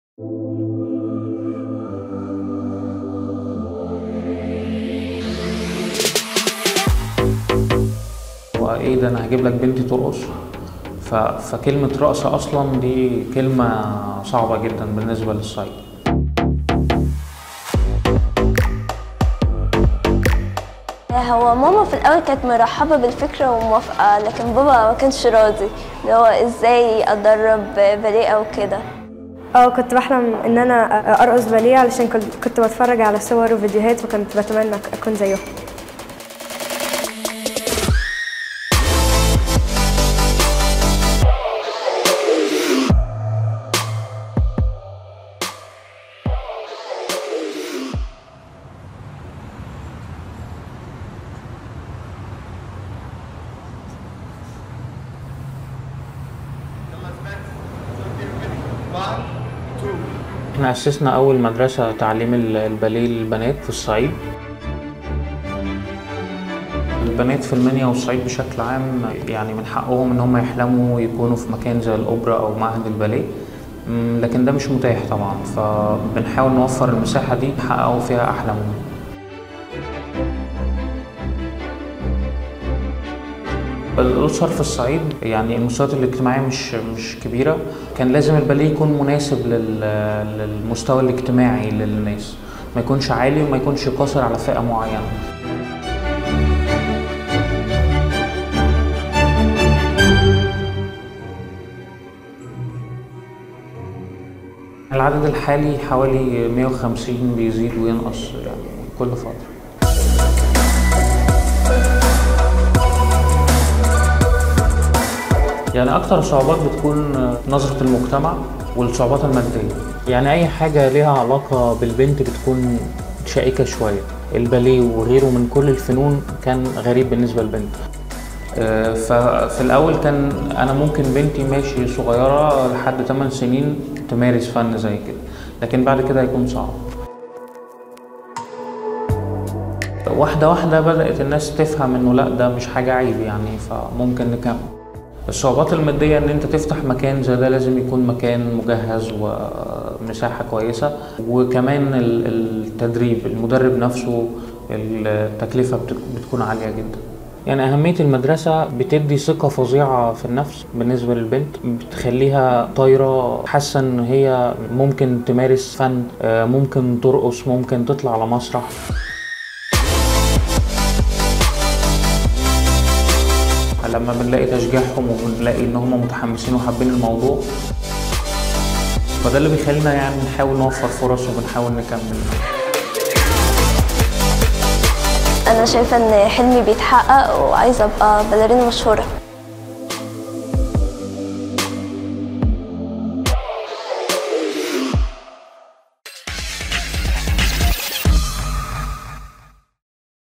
Dancing past limits and towards dreams
In one of Upper Egypt's most conservative areas, the idea of a girl dancing is almost unheard of. That's why Marco Adel opened a ballet school. He's determined to help these girls achieve their dreams.